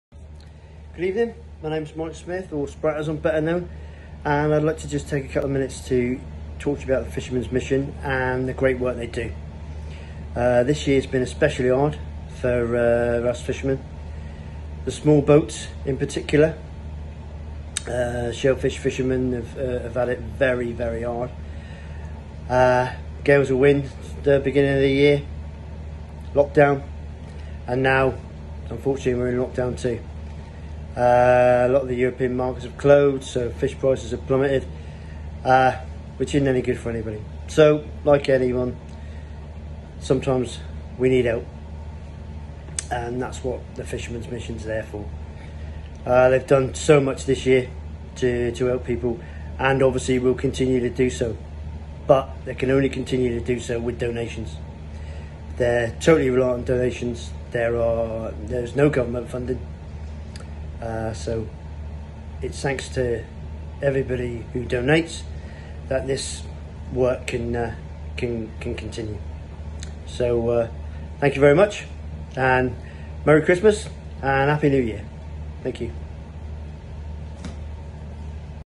Brixham Fisherman, 'Sprat,' Describes the Difficulties of 2020 For Fishermen
Fisherman, Mike Smith aka 'Sprat', explains how crucial the Fishermen's Mission's supporters are to the charity, and tell us how fishermen have struggled through storms, and Coronavirus, during 2020.